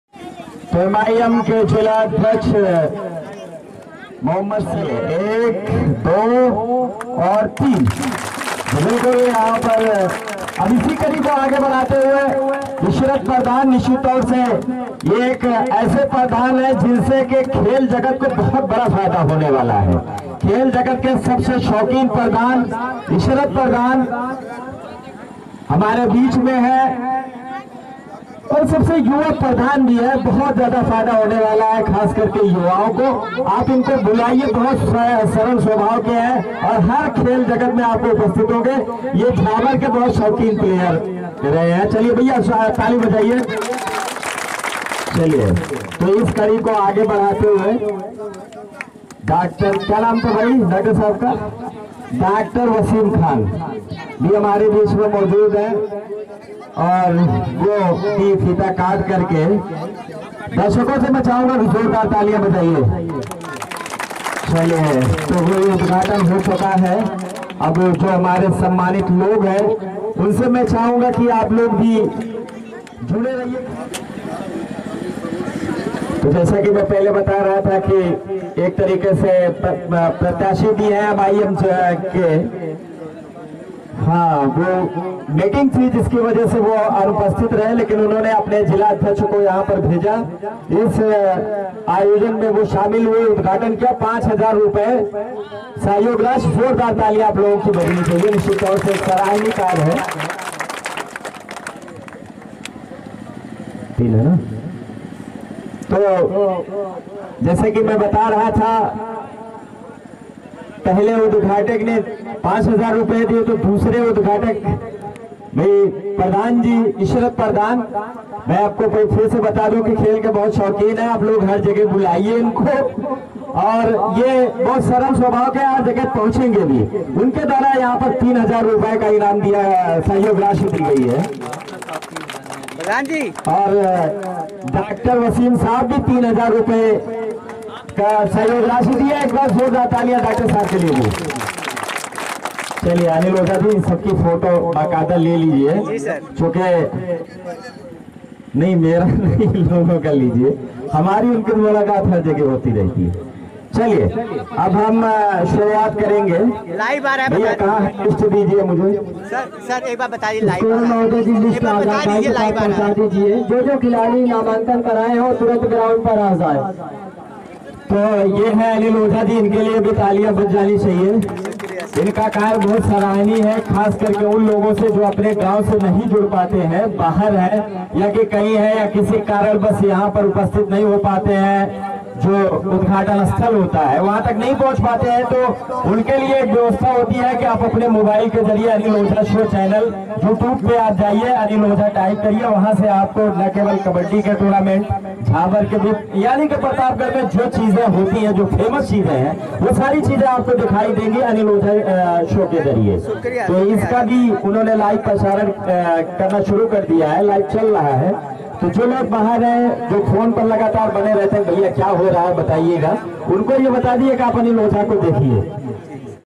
मेरी बेगारी : और कमेटी मालामाल
मैनहा लंबी कूद और 100 मीटर रेस का उद्घाटन समारोह जिसमे अनेक गणमान्य व्यक्ति मौजूद थे।
मेरी बेगारी : और कमेटी मालामाल @Anil Ojha Show